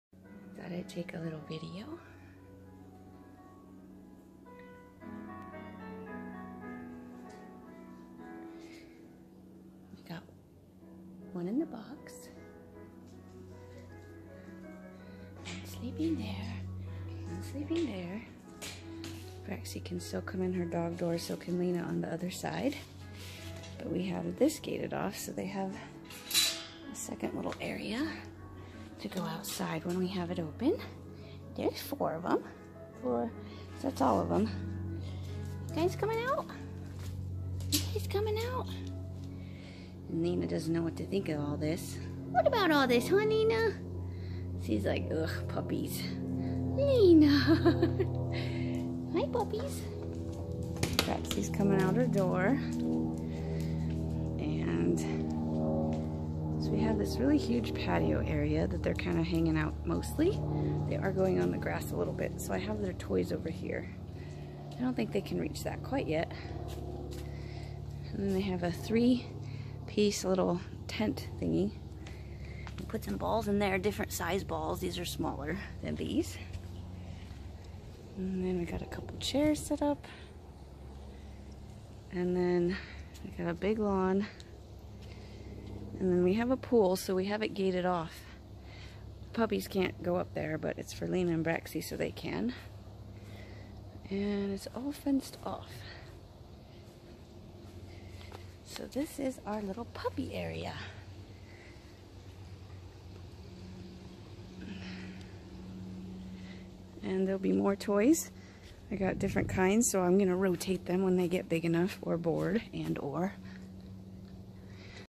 Eurasier puppies area 4 to 8 weeks old.
Here is their puppy area from four to eight weeks old inside and outside. We rotate and switch toys around as they grow. We also follow a program called Puppy culture for their best brain development!